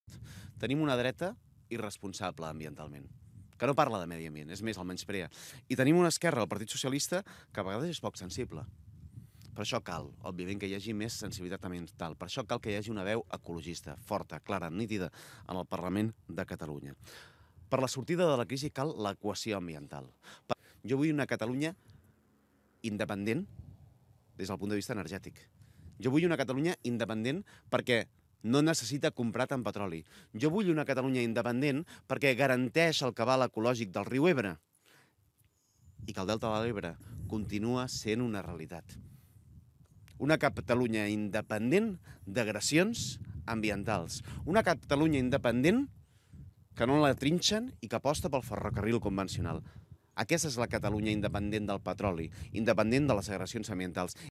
Herrera reivindica una "Catalunya independent energèticament i lliure d'agressions mediambientals"
El cap de llista d'ICV-EUiA, Joan Herrera, ha reivindicat avui el balanç de govern d'Entesa en defensa del medi ambient i en la lluita contra el canvi climàtic i ha subratllat el valor de l'ecologia per sortir de l'actual crisi econòmica. Herrera ha demanat "una Catalunya independent energèticament i lliure d'agressions mediambiental" i ha afirmat que la seva formació "és l'única força que aposta per la lluita contra el canvi climàtic". En aquest sentit, ha demanat el vot "a aquells que es preocupen i són sensibles  pel medi ambient".